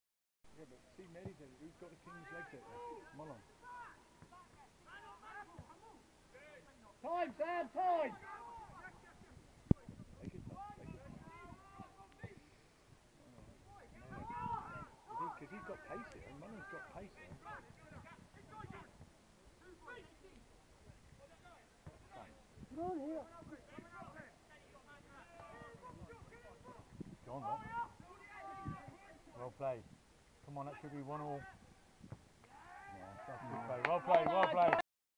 Bodmin Trophy Game ,Lifton 3 x 4 St Stephen Res after xtra time (3-3 at Full Time)
match highlights A Determined run down the left flank and a cross by Jordi Willmott to his Brother Reece who slots the ball home once he gets his feet right.